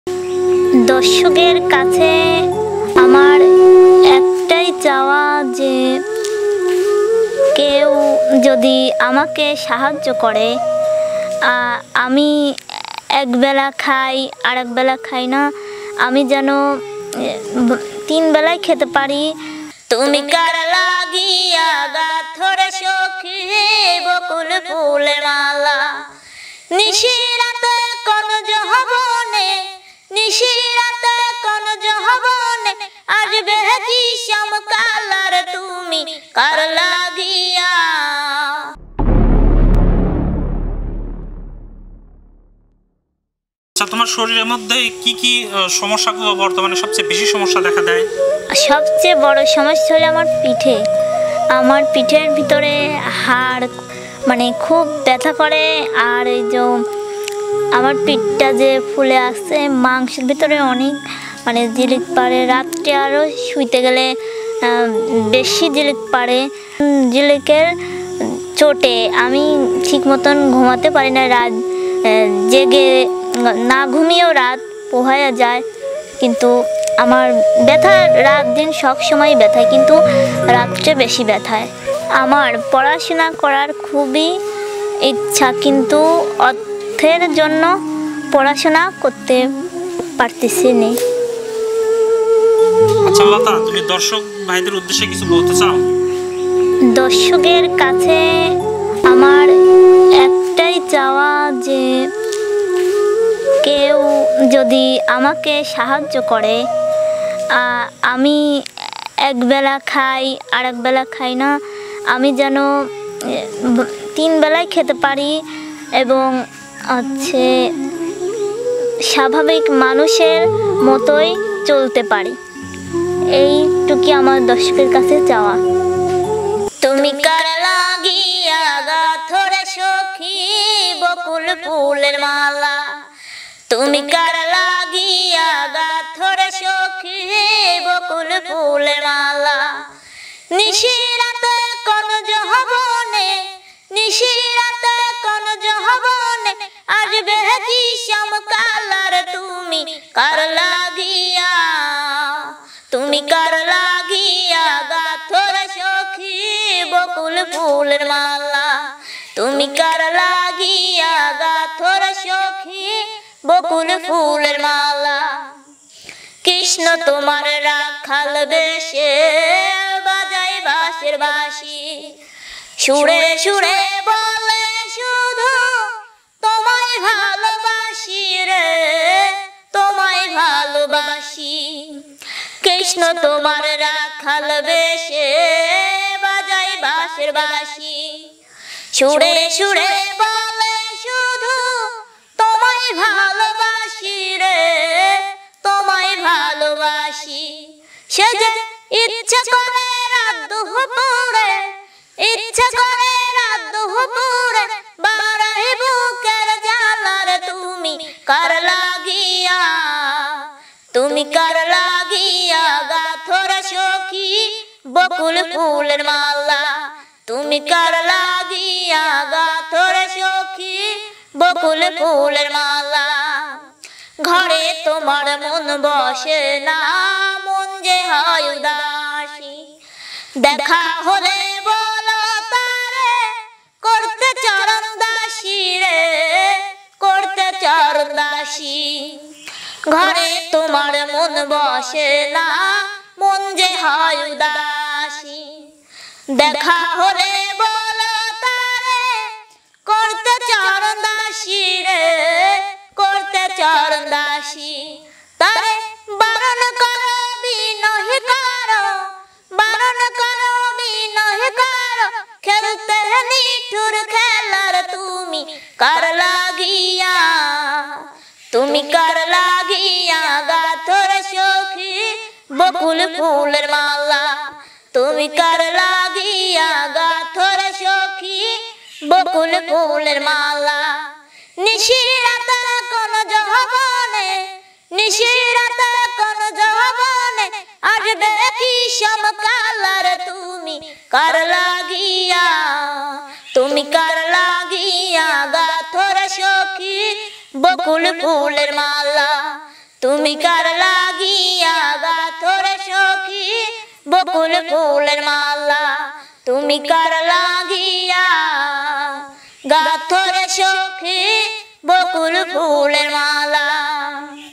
অসহায় প্রতিবন্ধী লতা জীবনের কষ্ট নিয়ে কষ্টের গান গাইলো | সবাইকে কাদিয়ে দিল | reduce sin | hb tv
অসহায় প্রতিবন্ধী লতার জীবনে অনেক কষ্ট। সবাই সহযোগিতা করুন তাকে। প্রয়োজনে যোগাযোগ করুন ০১৭০৫৩০১৩৭৮.
অসহায় প্রতিবন্ধী লতা বকুল ফুলের মালা নিয়ে যা চাইলো সবার কাছে | লতার জীবনের কষ্ট | reduce sin | hb tv